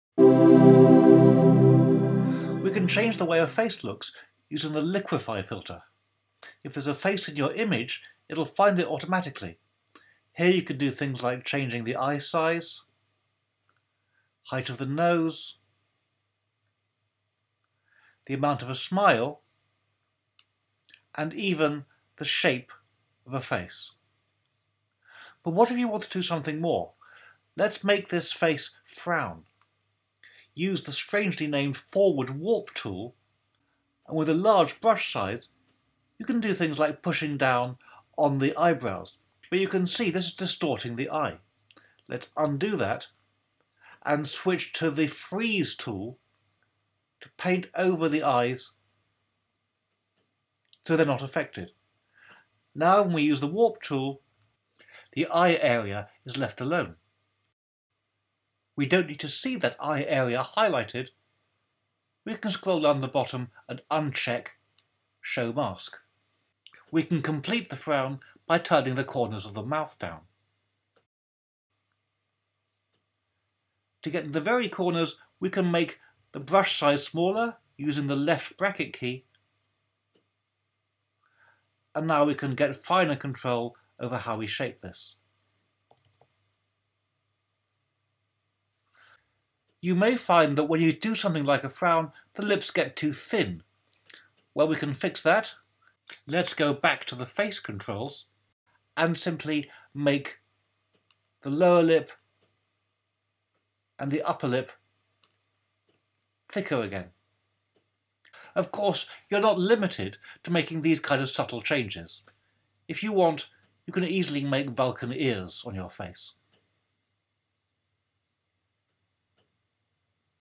Using the Liquify filter to change facial expressions
How to use Photoshop's Liquify filter to change not only the shape of a face, but the expression - all in just two minutes.